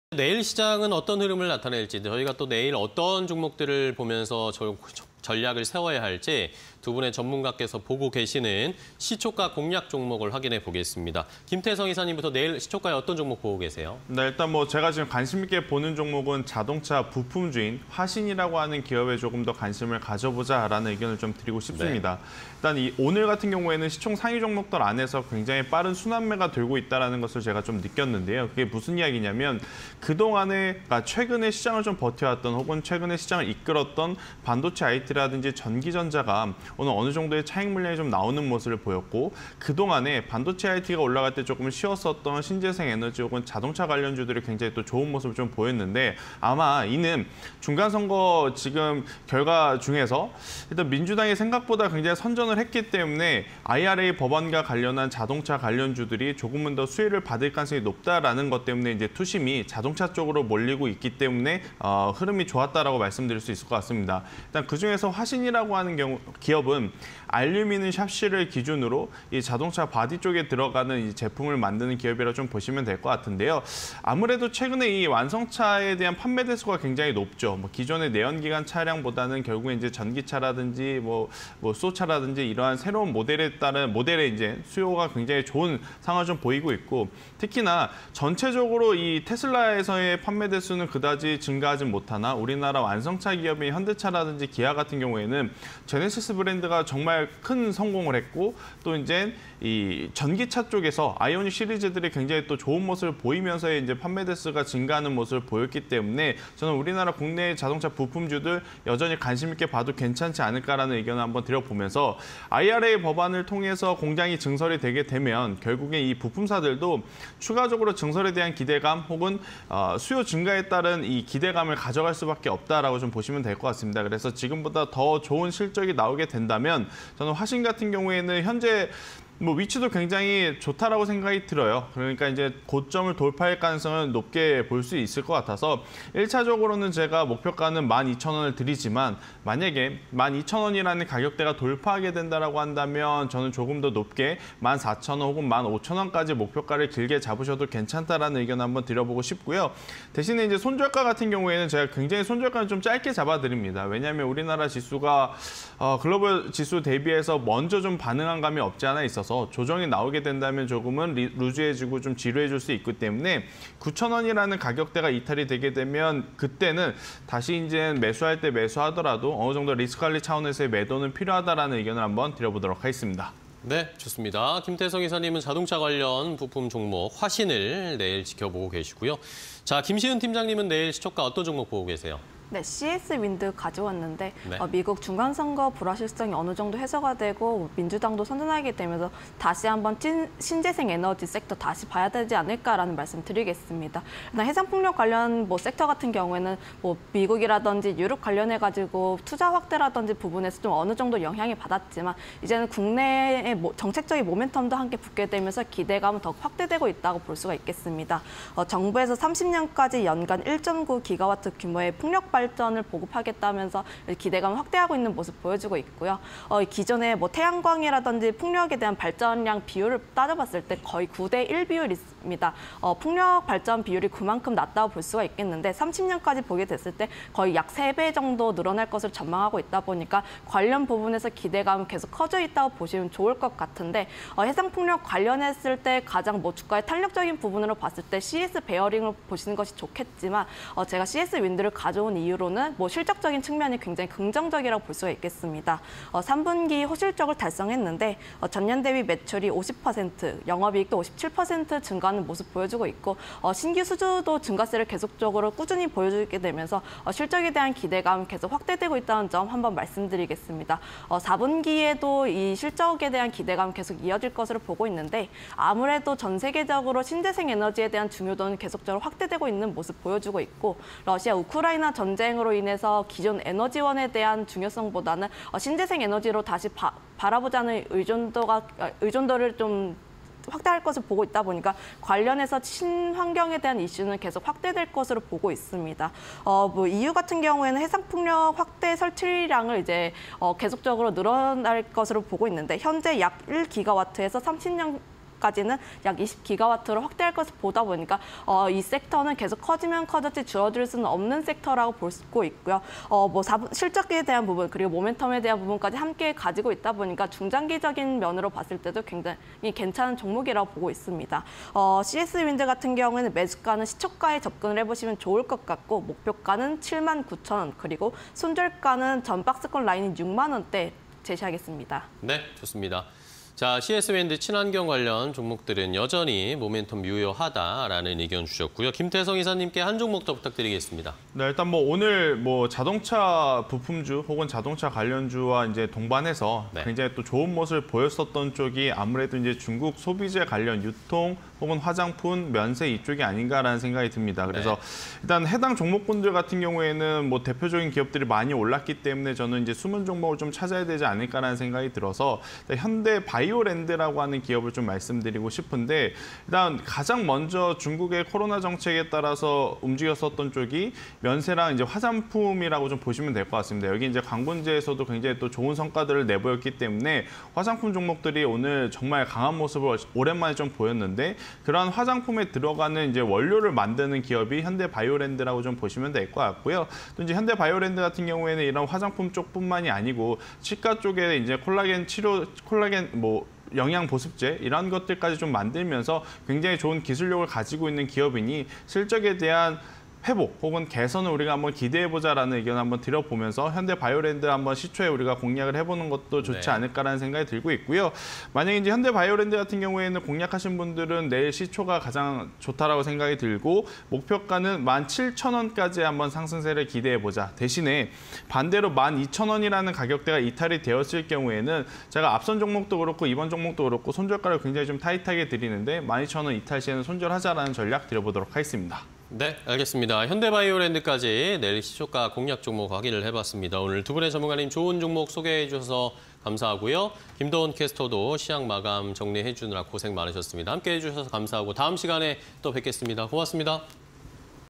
화신(010690), 씨에스윈드(112610), 현대바이오랜드(052260)_사가지 (20221114)
화신(010690)
- 자동차용 섀시ㆍ보디 제품 생산
- 美 OEM의 배터리 발주 시작… 구조적 수혜
- IRA 법안 기대… 자동차주 수급 유입 유효

씨에스윈드(112610)
- 풍력발전 목표 조정… 사업 속도 기대감 확대
- 3분기 호실적 기록… 영업이익 57.6% 증가
- 글로벌 수주 증가 등 4분기 이후 실적 성장 기대

현대바이오랜드(052260)
- 천연물 이용한 의약품ㆍ화장품 등 제조
- 중국 완화 정책에 따른 수혜주
- 지속적 기술 개발 통한 외형 확대 기대